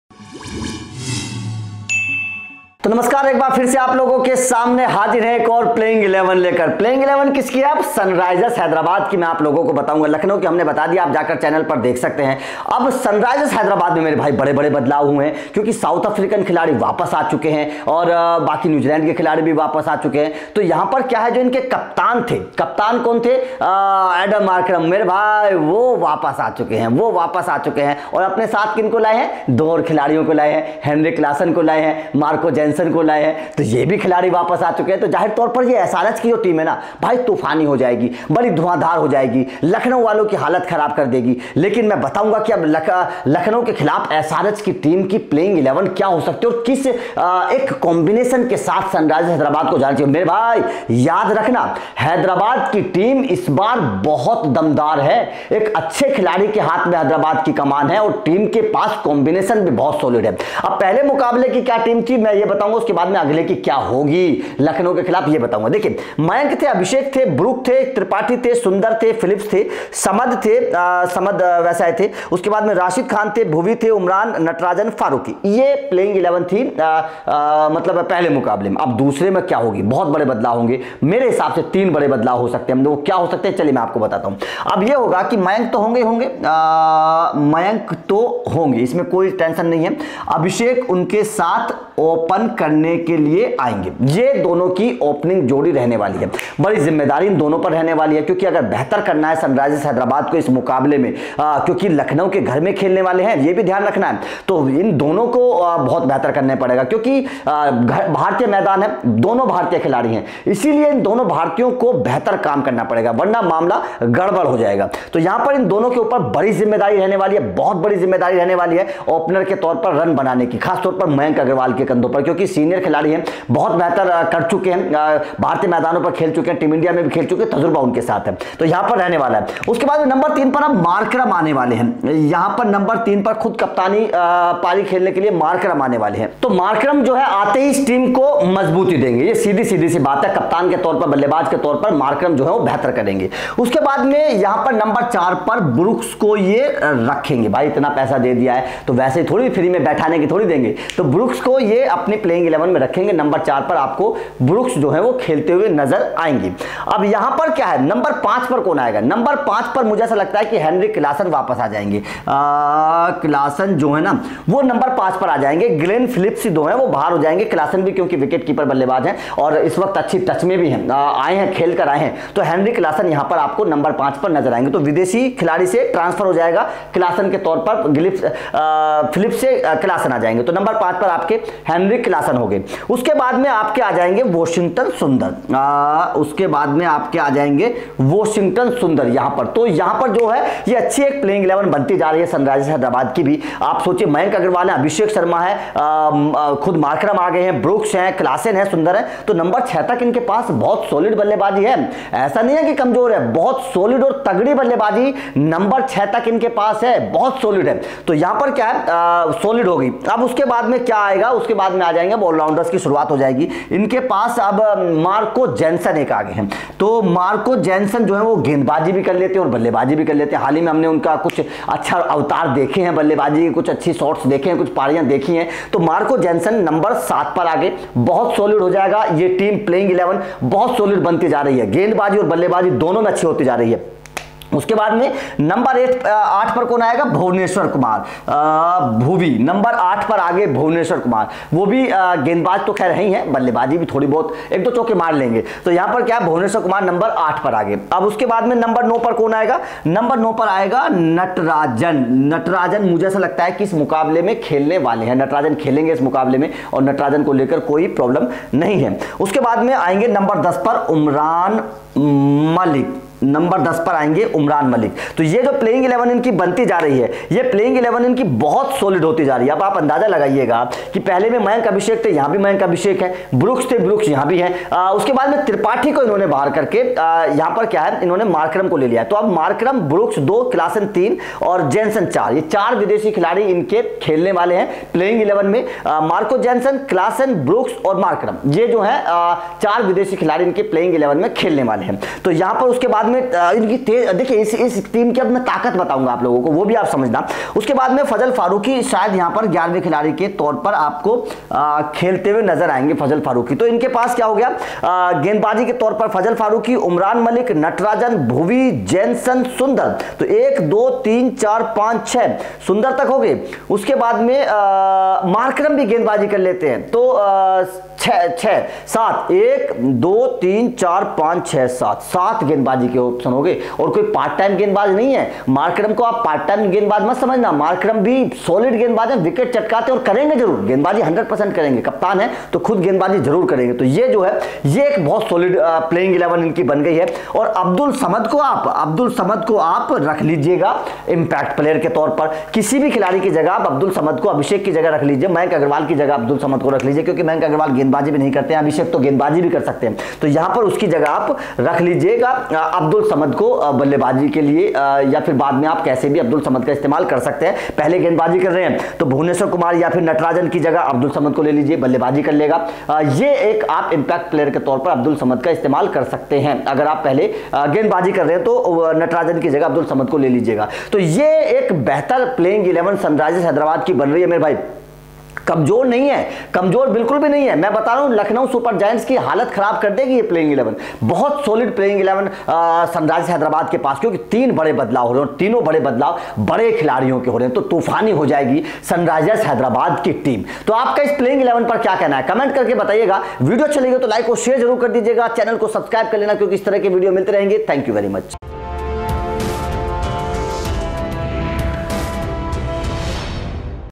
LSG vs SRH Dream Playing 11 | IPL 2023 | Lucknow vs Hyderabad Match
दोस्तों आज के इस वीडियो मैं हम देखने वाले हैं Lucknow Super Giants और Sunrisers Hyderabad  के Final Playing 11, Pitch Report, प्लेयर Updates, Players Match UP’s, और Team Prediction, और Fantasy Team के बारे मैं।

DON'T MISS :-